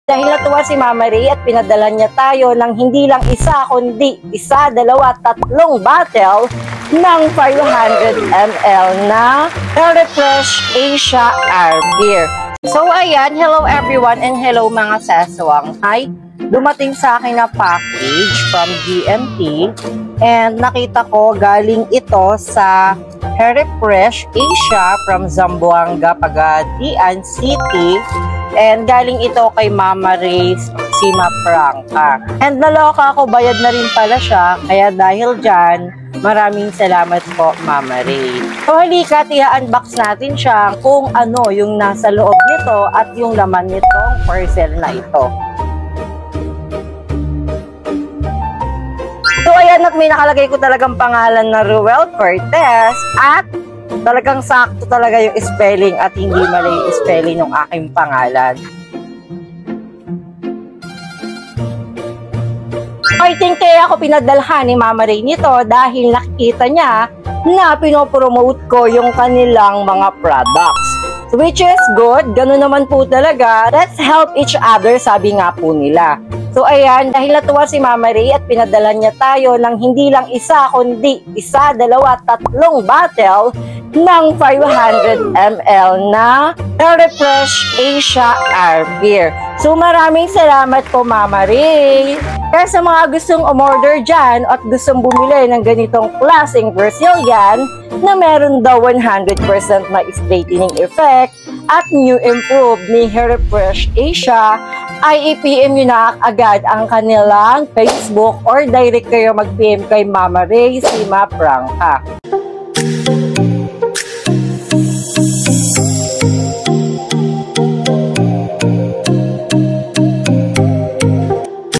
PINADALHAN AKO NG HAIR REFRESH ASIA BRAZILIAN R’PAIR NG CEO
DAHIL NAKITA NG COMPANY NG HAIR REFRESH ASIA ANG ATING MGA MAGAGANDANG VLOGS AY NATUWA SILA AT PINADALHAN NILA AKO NG KANILA BRAZILLIAN R’PAIR . THANK YOU SO MUCH MAMA REY PAULIN CIMAFRANCA ❣️❣️❣️

HAIR REFRESH ASIA BRAZILIAN R’PAIR
THE ULTIMATE HAIRCARE SYSTEM
3in1 NEW IMPROVED ODORLESS
100% STRAIGHTENING EFFECT
FOR CHEMICALLY AND TREATED HAIR.